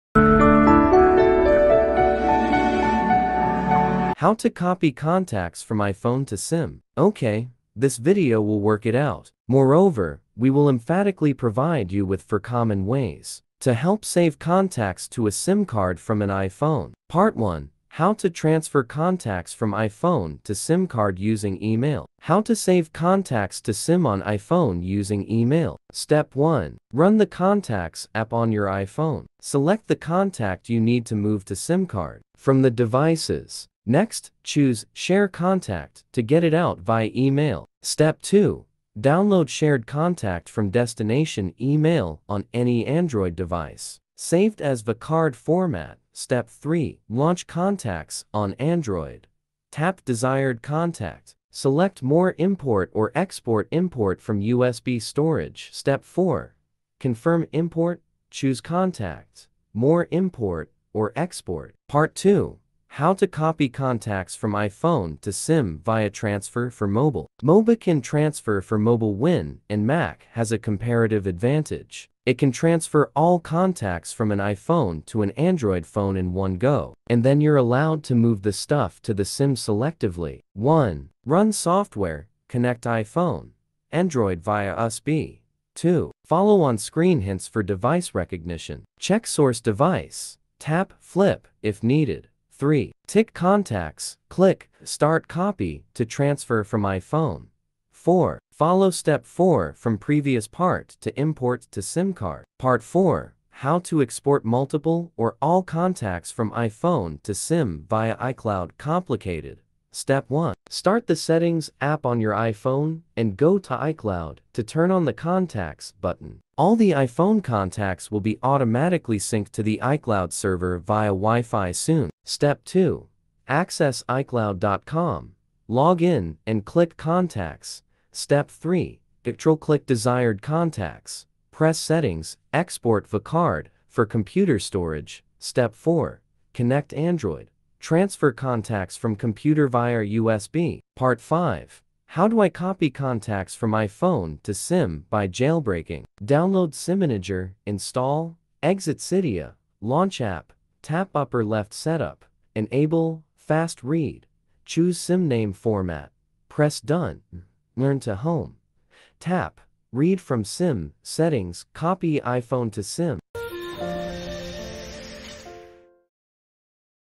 How to Transfer Contacts from iPhone to SIM Card Easily & Quickly
Still don't know how to copy contacts from iPhone to SIM after trying it many times? Look over here! This video is intended to give you the confidence to save contacts to SIM card from iPhone efficiently.